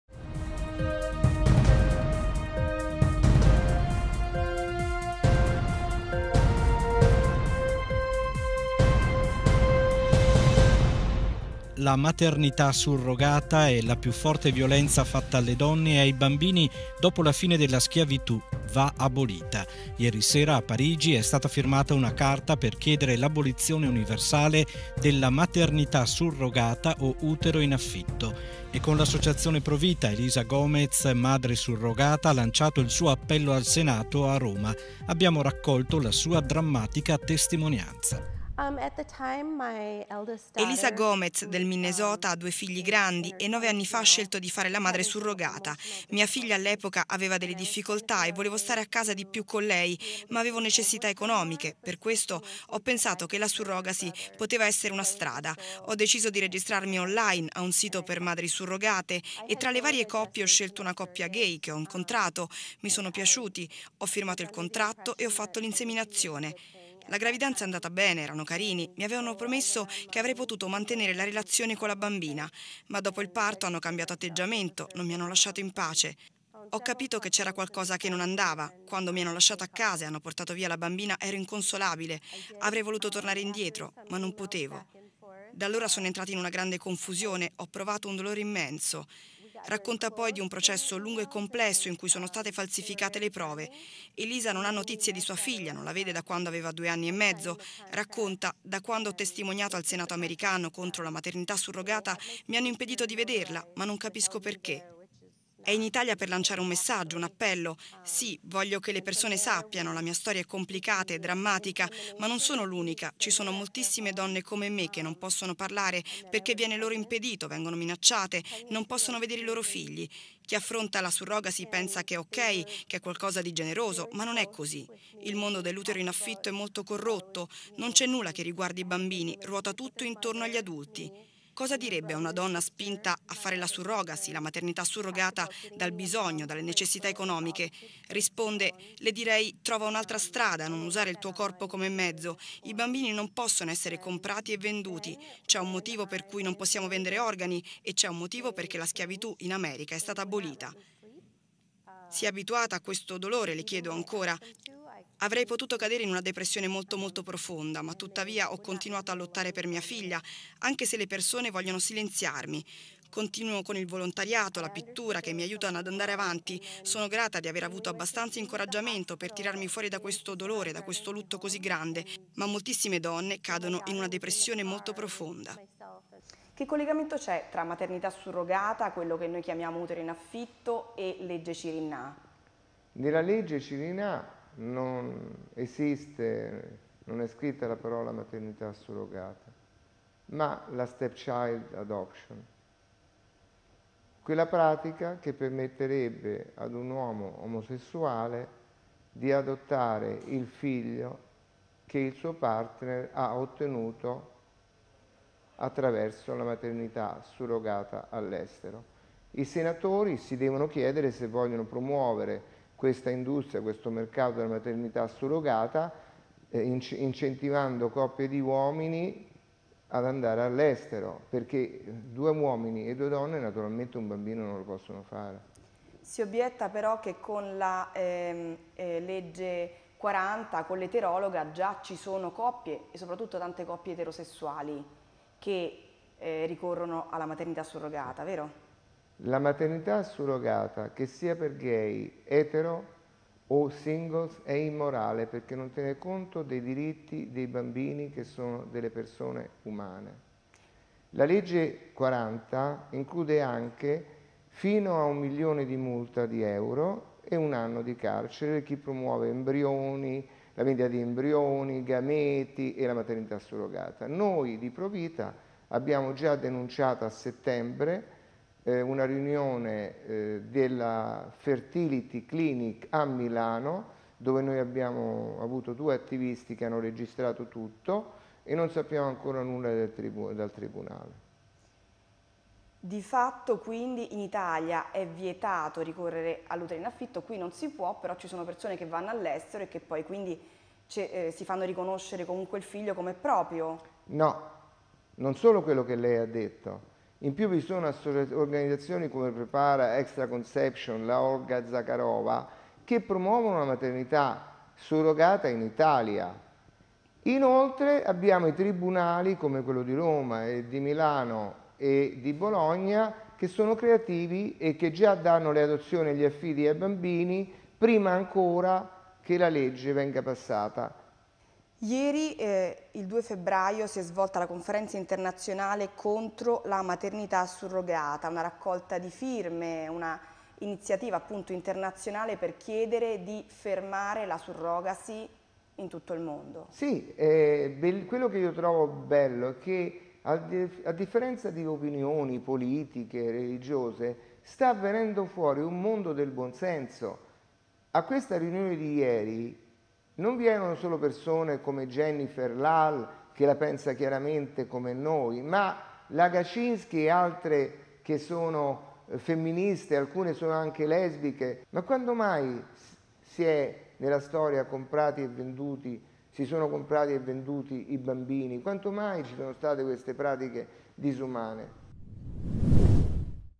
L’utero in affitto è schiavitù
ProVita chiede l’abolizione universale della maternità surrogata (utero in affitto), quale nuova forma di schiavitù.
Ecco l’appello al Senato di Elisa Gomez!